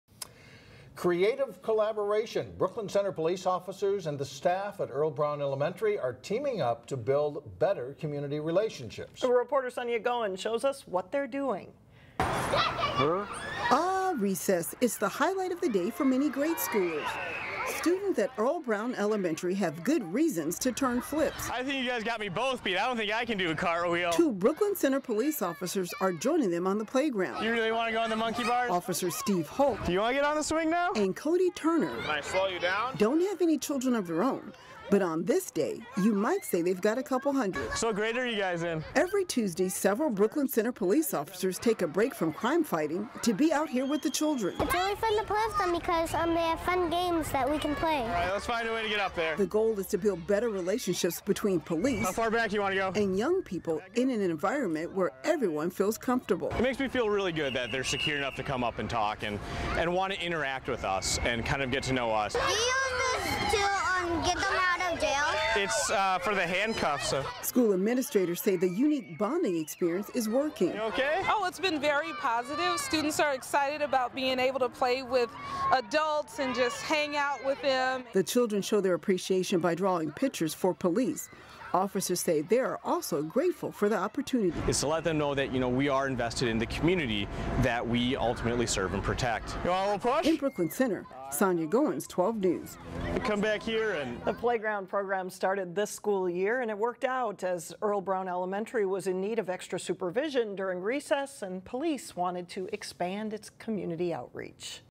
Earle Brown students bond with police at recess
First and second-grade students at Earle Brown Elementary have a good reason to get excited about recess. That's because two Brooklyn Center police officers took time out of their shift and joined them on the playground.
Officers Steve Holt and Cody Turner don't have any children of their own, but on this day you might say they've got a couple hundred.
"It makes me feel really good that they're secure enough to come up and talk and want to interact with us and kind of get to know us," said Holt.
Every Tuesday, several Brooklyn Center officers take a break from crime fighting to be out on the playground. The effort started this school year as a way for Brooklyn Center police to expand their community outreach. It worked out because Earle Brown Elementary was also in need of extra supervision during recess.
"It's really fun to play with them," said first-grader Adela Law. "They have fun games that we can play."
School administrators say the unique bonding experience is working.
"It's been very positive, students are excited about being able to play with adults and just hang out with them," said Earle Brown Principal Callie Lalugba.
The children show their appreciation by drawing pictures for the police. Officers say they are also grateful for the opportunity.
"It's to let them know that we are invested in the community that we ultimately serve and protect," said Holt.